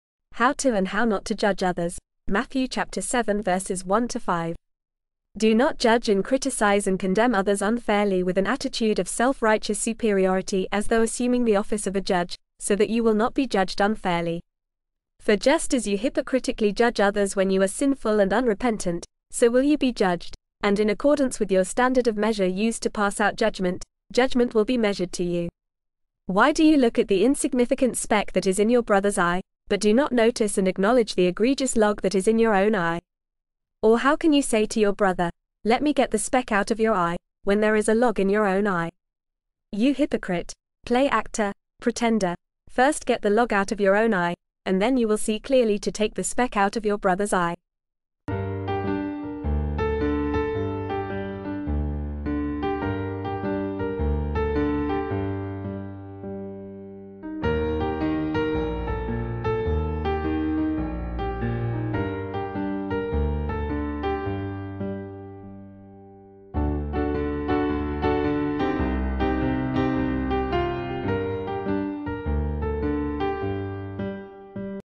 How To and How Not To Judge Others Matthew 7:1-5
Do not judge and criticize and condemn others unfairly with an attitude of self-righteous superiority as though assuming the office of a judge, so that you will not be judged unfairly.

For just as you hypocritically judge others when you are sinful and unrepentant, so will you be judged; and in accordance with your standard of measure used to pass out judgment, judgment will be measured to you.

Why do you look at the insignificant speck that is in your brother’s eye, but do not notice and acknowledge the egregious log that is in your own eye?

Or how can you say to your brother, ‘Let me get the speck out of your eye,’ when there is a log in your own eye?

You hypocrite, play-actor, pretender, first get the log out of your own eye, and then you will see clearly to take the speck out of your brother’s eye.

Our Network Partners Links:
* Video Creation Partners:
* Stock Photos, Editorial Photos, Illustrations, Videos, Audio, Free Photos